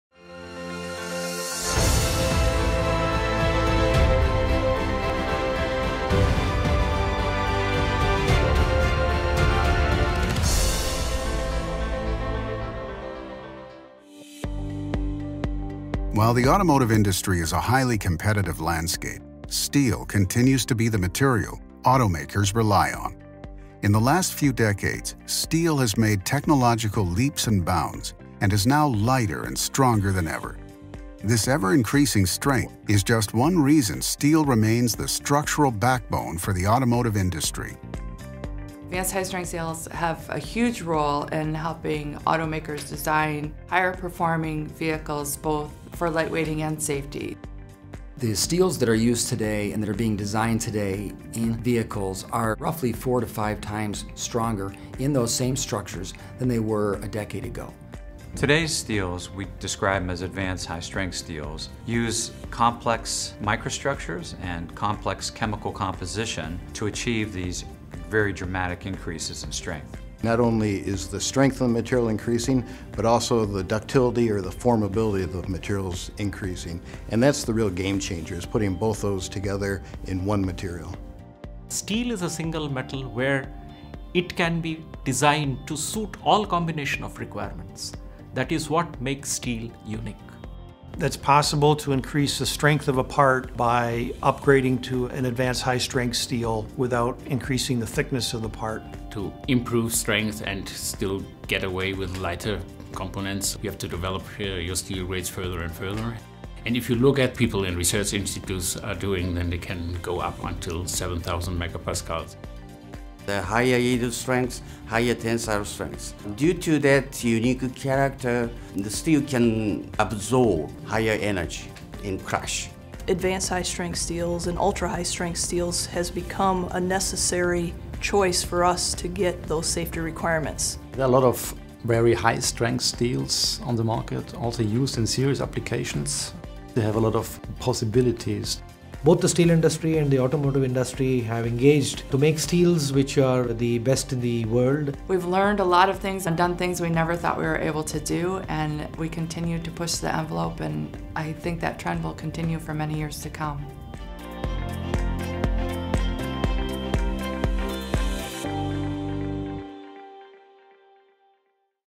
[POSCO Newsroom] What Does a Car’s Lightweight and Safety Depend On?
A car’s quality depends on its backbone, the quality of steel.
Steel is in high demand in the automotive industry.
Its competence is ever increasing in terms of weight and strength.

Today’s steel, known as advanced high-strength steels have been developed
along with the advancement of automotive industry.
Let’s find out more about how the quality of steel is evolving.

자동차 시장이 발전하면서
자동차 구조물을 형성하는 강철의 제질 역시 성장해왔습니다.
철강 산업의 발전 과정을 자세히 알아볼까요?